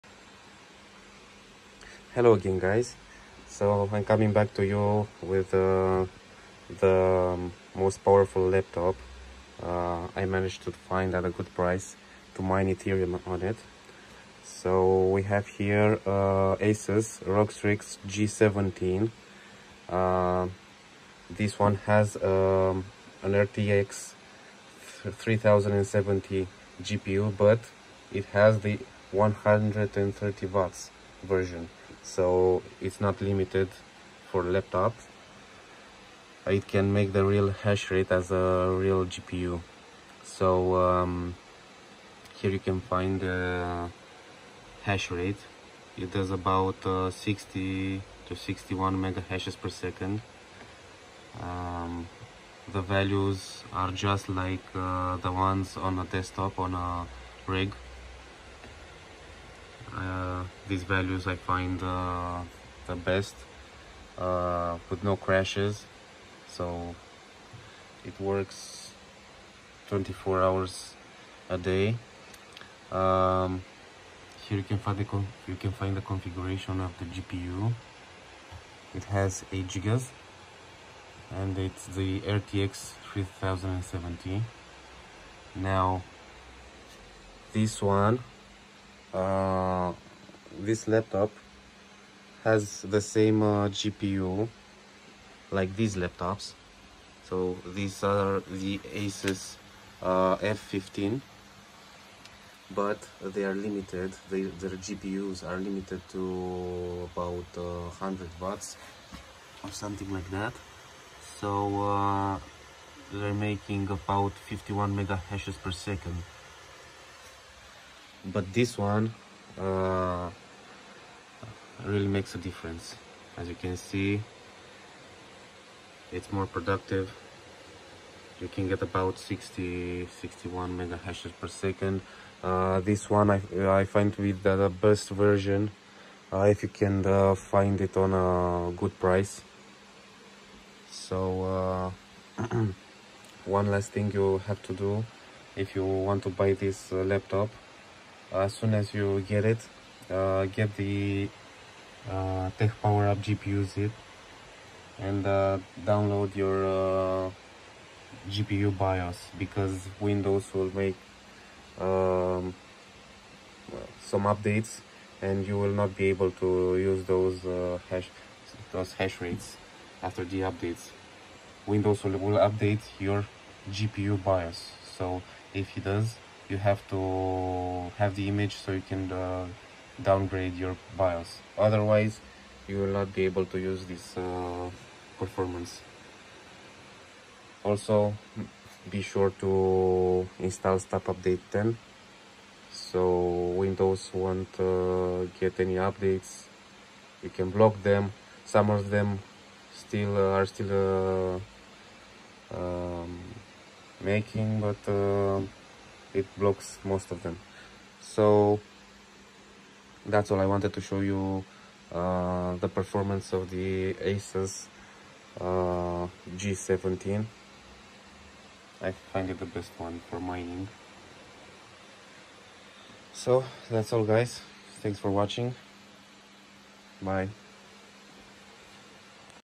Mining ETH on Asus Rog Strix G17 with RTX 3070 8GB Laptop GPU 130W
Mining Ethereum on Asus Rog Strix G17 equiped with a Nvidia RTX 3080 8GB GPU 130W Version.
Hashrate is about 65Mh/s, with the maximum memory OC.
It has the same performance as RTX 3080 8Gb and 16Gb, so I find it the best laptop GPU for mining crypto.

Laptop configuration:

ASUS ROG Strix G17 G713QR Gaming Laptop
CPU: AMD Ryzen™ 7 5800H up to 4.40 GHz
Display: 17.3", Full HD, 300Hz
Memory: 16GB RAM
Storage: 1TB SSD
Dedicated GPU: Nvidia RTX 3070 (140W TGP)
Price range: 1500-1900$

Please check my other videos with crypto mining on different gaming laptops, reviews, hashrates, settings, drivers, issues and more.